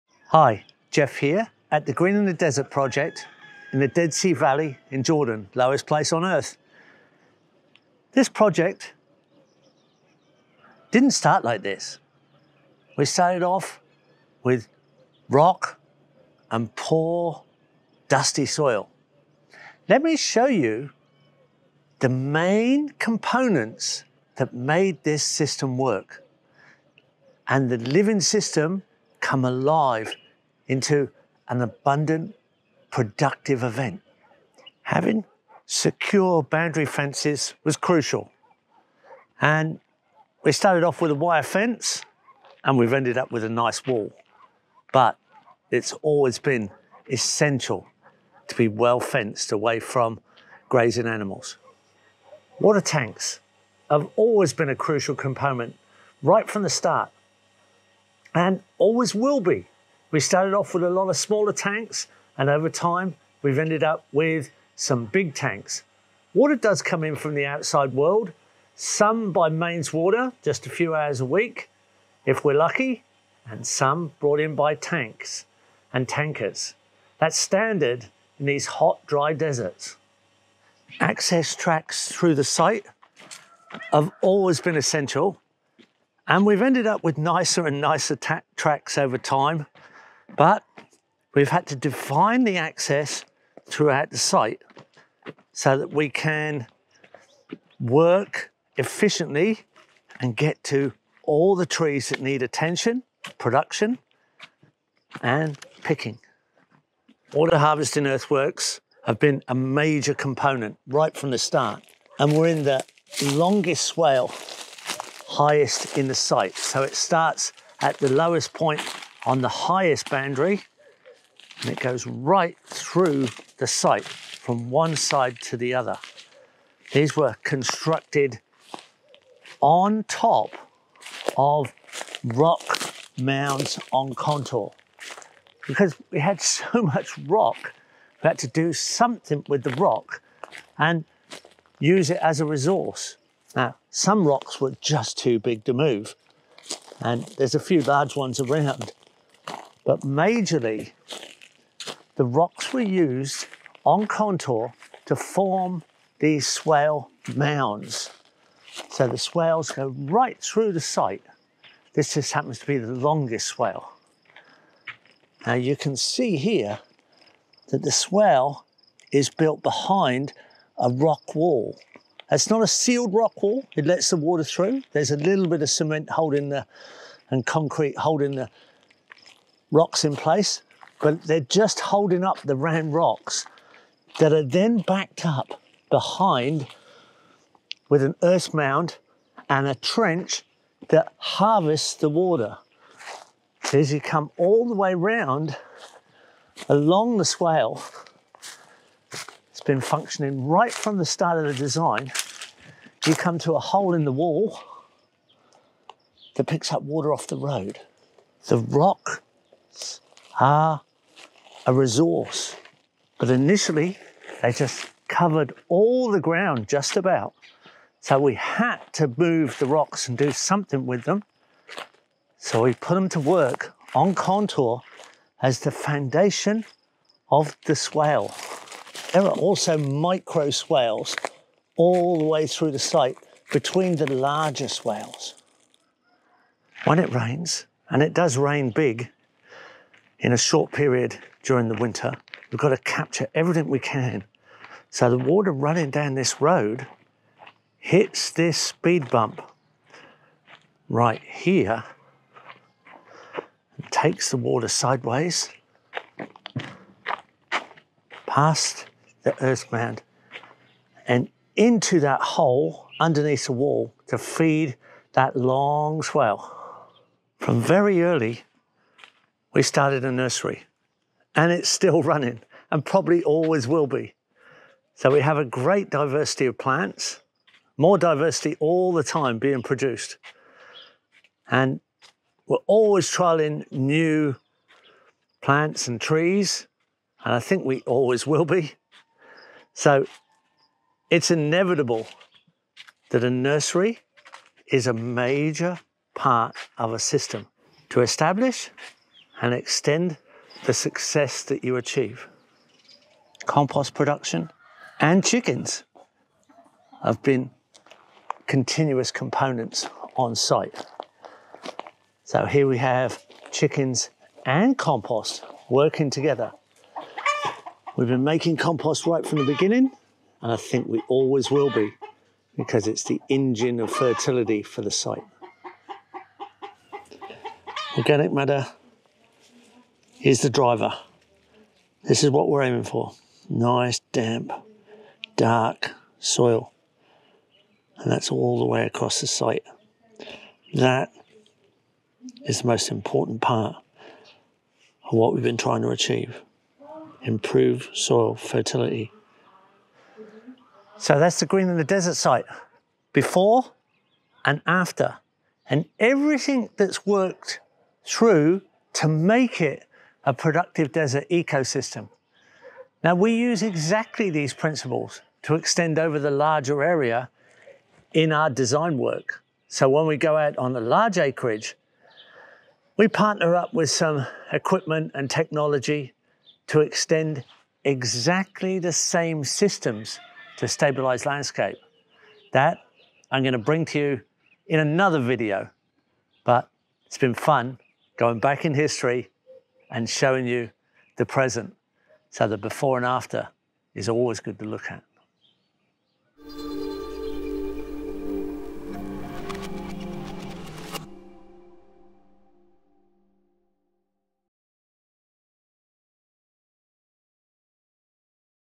How We Transformed the Desert
and to learn more about the Greening the Desert Project,

Join Geoff at the Greening the Desert Project in Jordan’s Dead Sea Valley to discover how this once barren, rocky landscape was transformed into a thriving permaculture oasis.

Key Takeaways:

- Secure fencing was essential to protect the site from grazing animals, starting with a wire fence and evolving into sturdy rock walls.

- Water tanks and earthworks played a crucial role in capturing and storing scarce water resources in this dry climate. Swales built on rock mounds collect rainwater, making every drop count.

- Well-defined tracks and pathways improved access throughout the site, facilitating maintenance and harvest operations.

- Composting and organic matter have been key to improving soil health, turning dry, lifeless soil into rich, productive ground that supports diverse plant life.

- A nursery was established early on and remains vital for continuously expanding the diversity of plants and trees on site.

- The principles used at the Greening the Desert Project are scalable, providing a model for restoring degraded land on a larger scale.

► And most importantly, enjoy your permaculture journey!